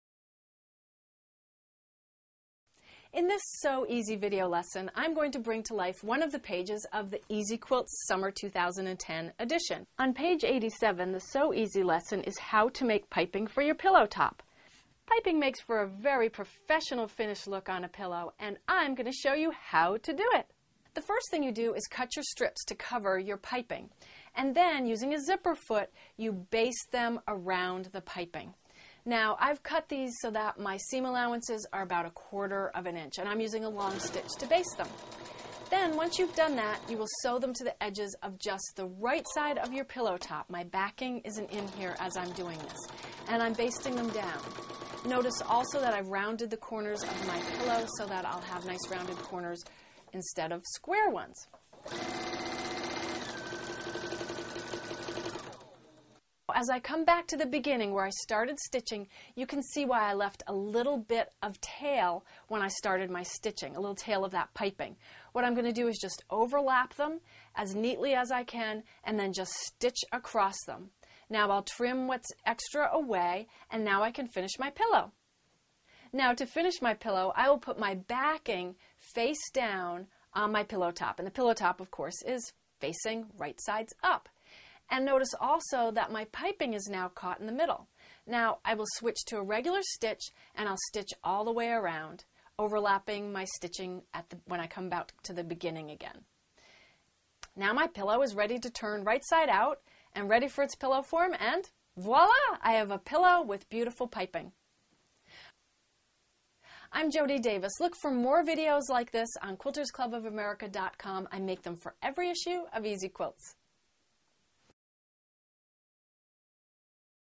Sew Easy Lesson: Piping for Pillows
This quilting video will show you how to make a quilt piping for a pillow. This classical finish is perfect for making your pillow look put together.